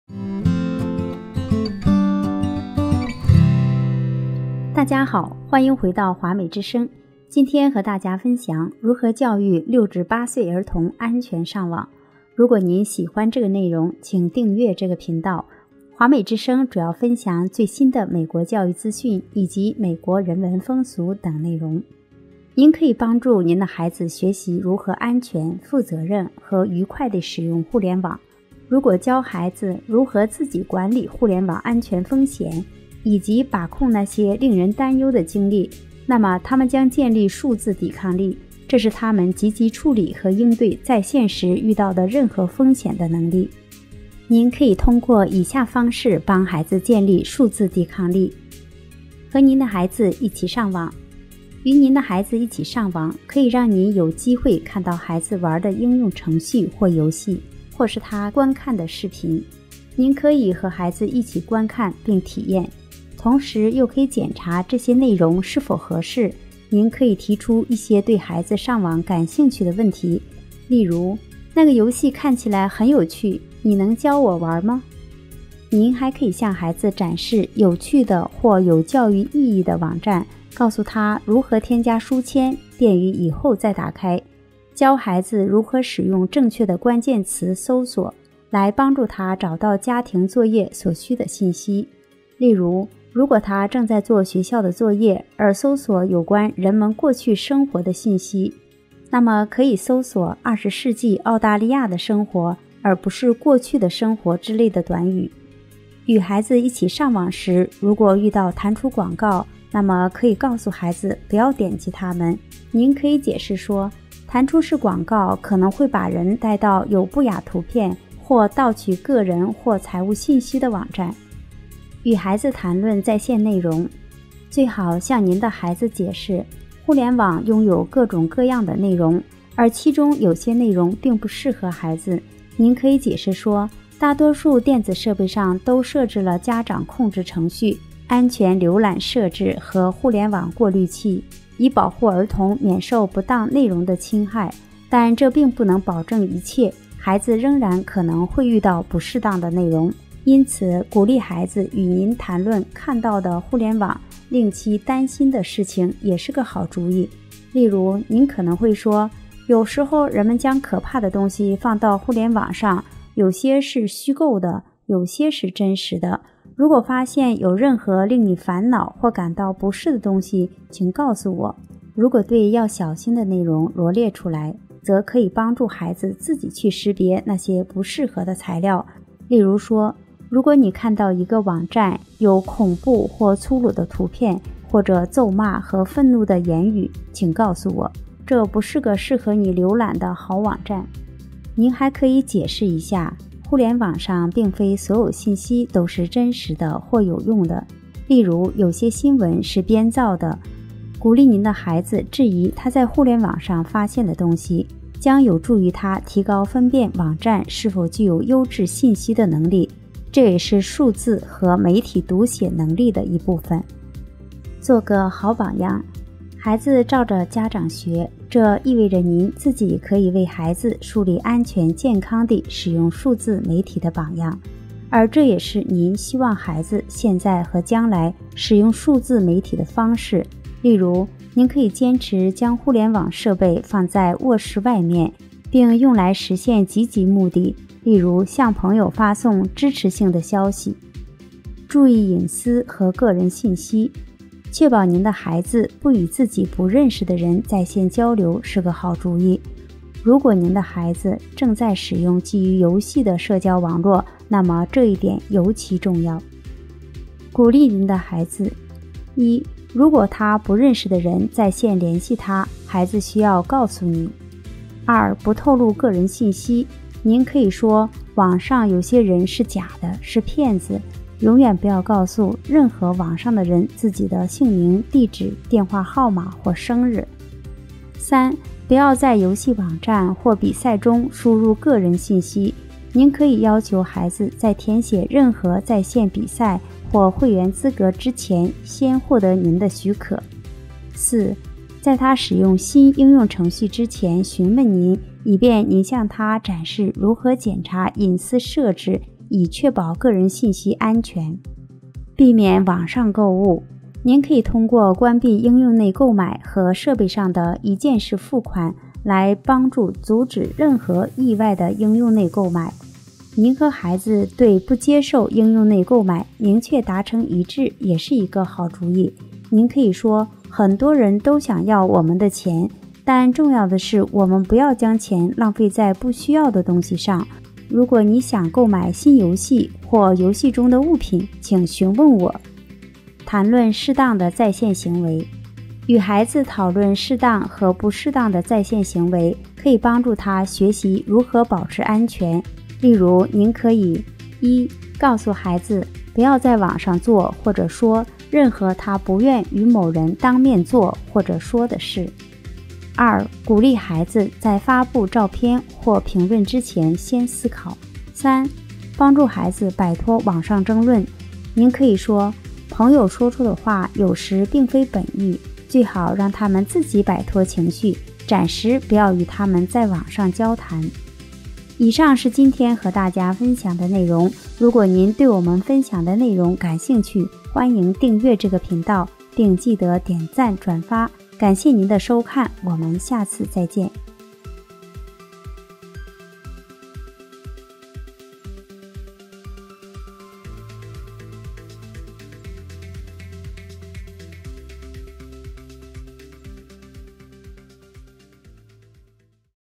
【华美之声】如何教育六至八歲兒童安全上網
鼓勵您的孩子：
1）如果他不認識的人在線聯繫他，孩子需要要告訴你。
2）不透露個人信息。您可以說：「網上有些人是假的，是騙子。永遠不要告訴任何網上的人自己的姓名、地址、電話號碼或生日。」
3）不要在遊戲網站或比賽中輸入個人信息。您可以要求孩子在填寫任何在線比賽或會員資格之前先獲得您的許可。
4）在他使用新應用程序之前詢問您，以便您向他展示如何檢查隱私設置以確保個人信息安全。

欢迎居住在美国东部大华府地区的朋友收听WQER-FM96.7 全中文电台。